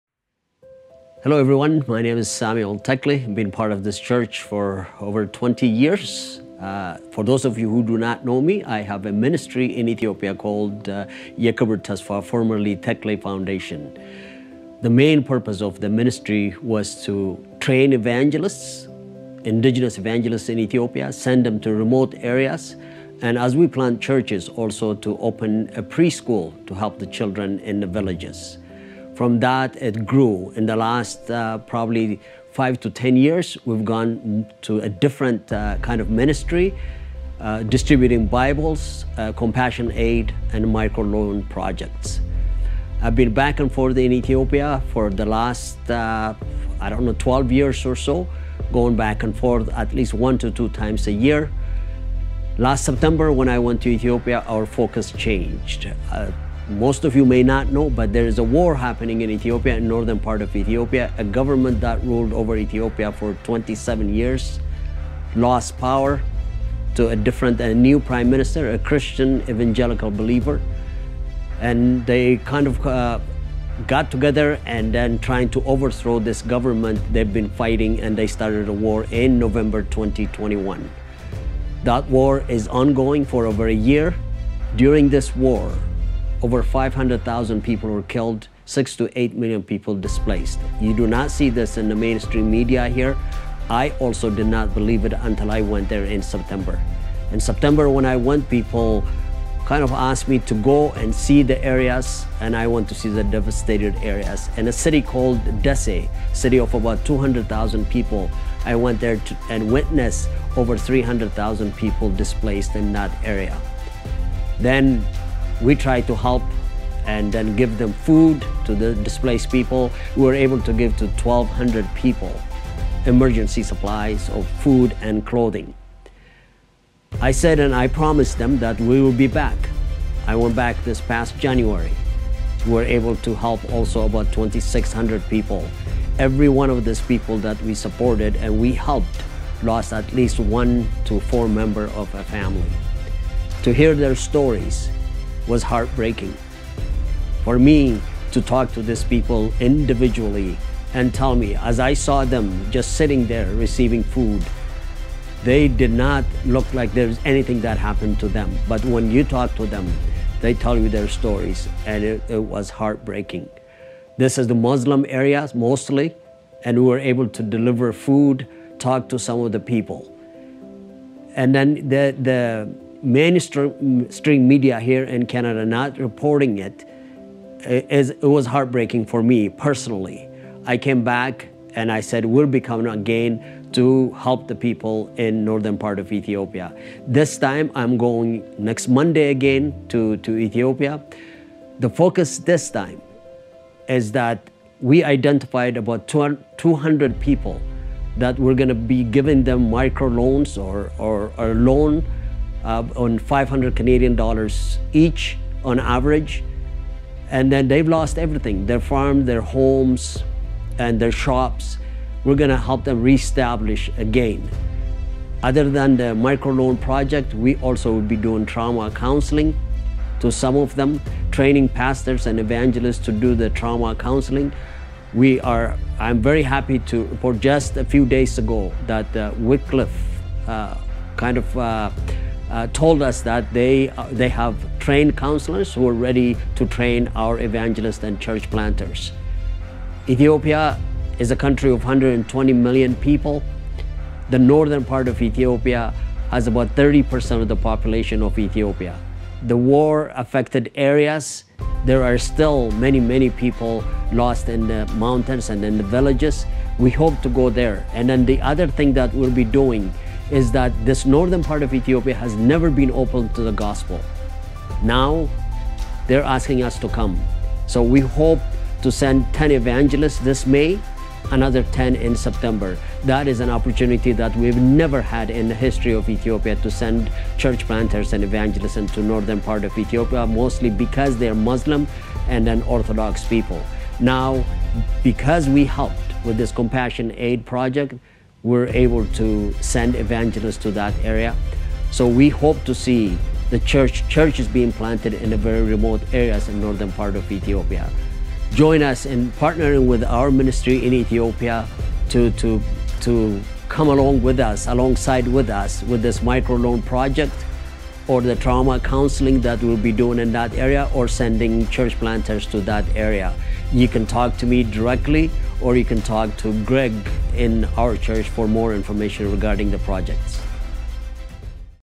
Sam Teckley Ethiopia Update May 8
Sam gives an update about what's going on in Ethiopia and his upcoming mission to help.